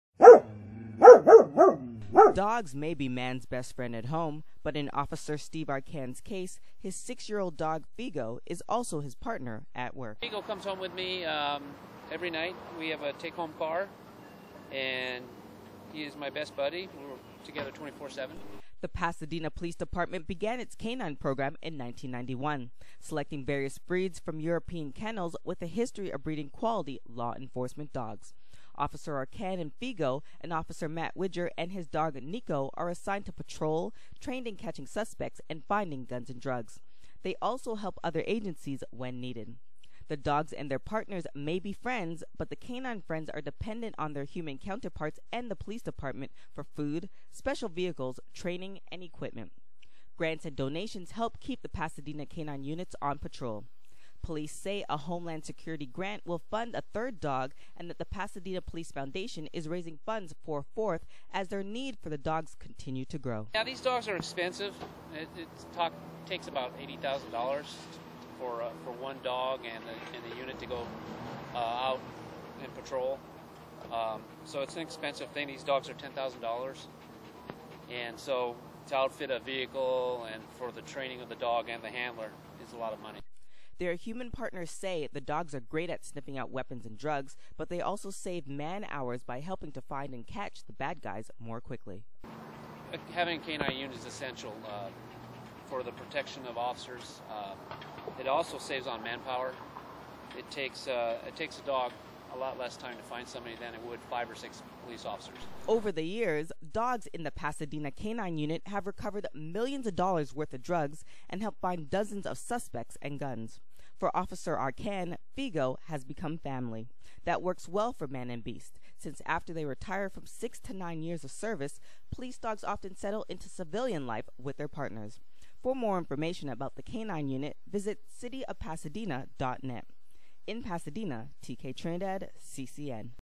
Man's Best Work Friend- Pasadena Police Department K-9 Unit
The Pasadena police department began its canine program in 1991, selecting various breeds from European kennels with a history of breeding quality law enforcement dogs.  Officer Arcand and Figo and Officer Matt Widger and his dog Nico are assigned to patrol, trained in catching suspects and finding guns and drugs. They also help other agencies when needed.  The dogs and their partners may be friends, but the canine friends are dependent on their human counterpoints and the police department for food, special vehicles, training, and equipment.  Grants and donations help keep the Pasadena K-9 units on patrol.  Police say a homeland security grant will fund a third dog, and that the Pasadena police foundation is raising funds for a fourth as their need for the dogs continues to grow.